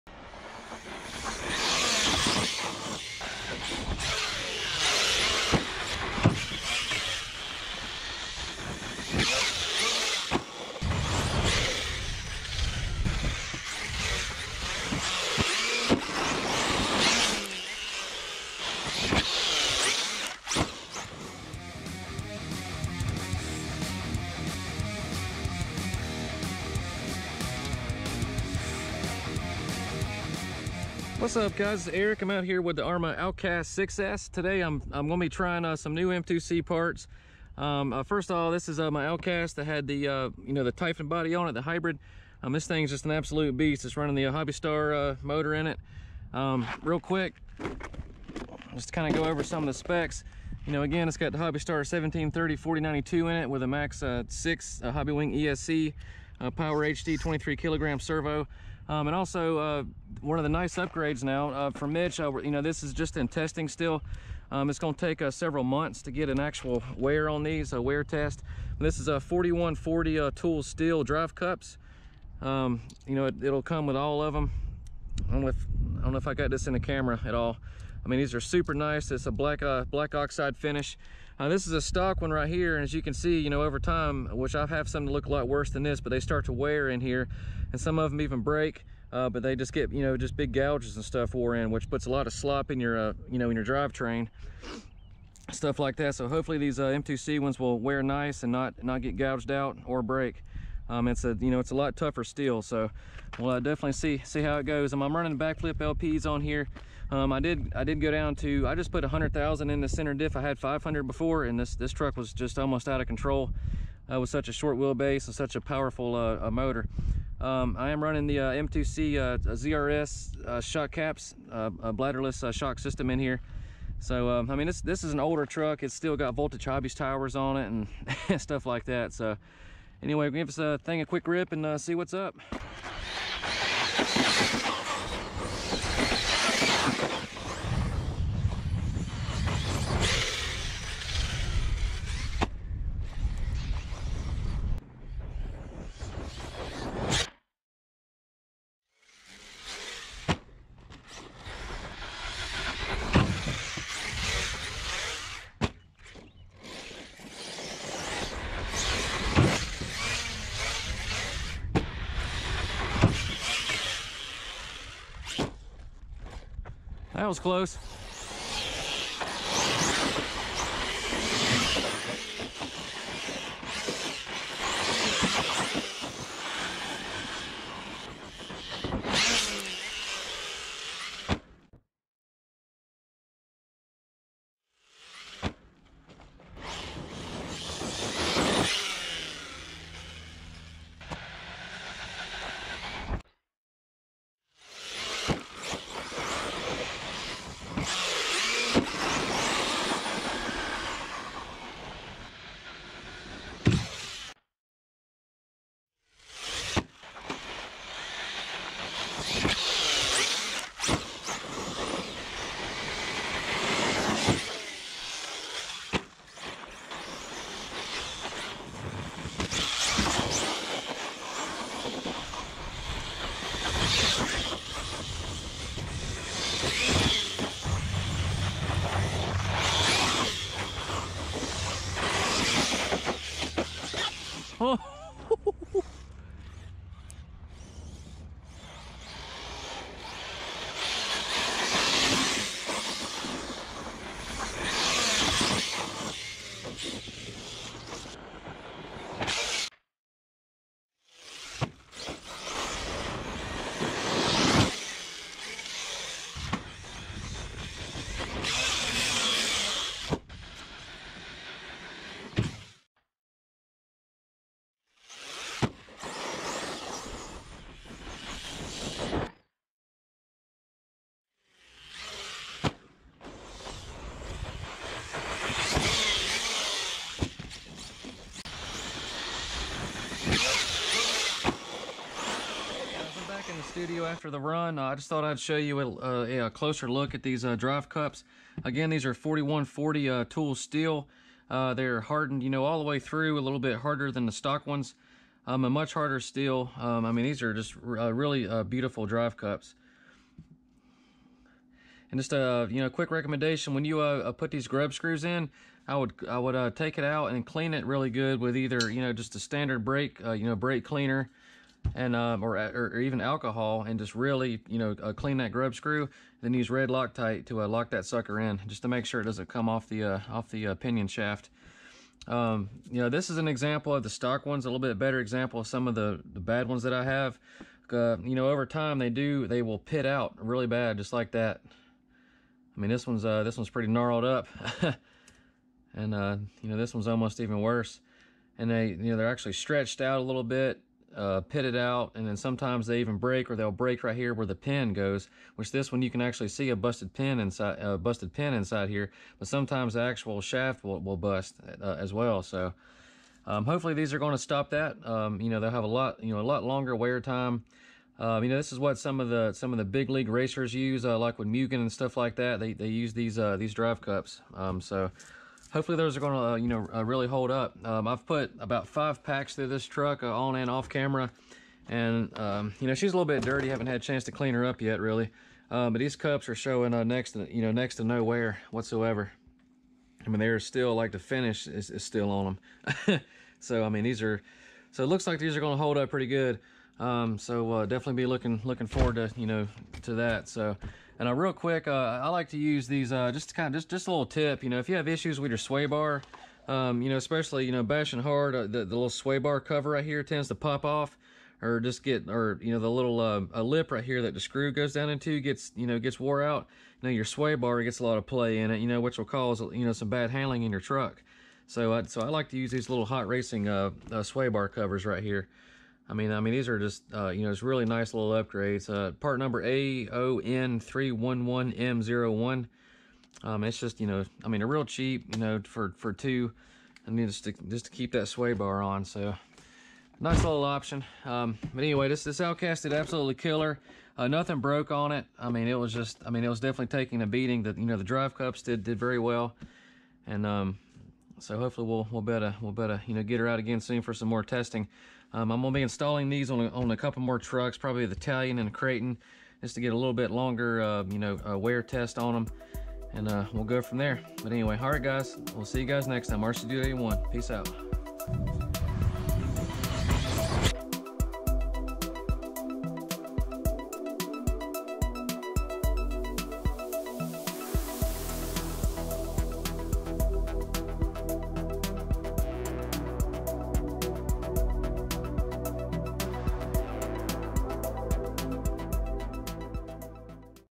Arrma Outcast 6s "GONE BALLISTIC!" -NEW M2C hardened steel Drivecups/Outdrives test!
Out for a beatdown with the trusty ole Arrma Outcast 6s! Testing some new 4140 tool steel M2C drive cups, all the parts did killer with virtually no signs of wear after 5 packs! These will take a little more time for an accuate wear test but so far, so good! The truck did amazing, no breaks other than a slightly bent front/center driveshaft.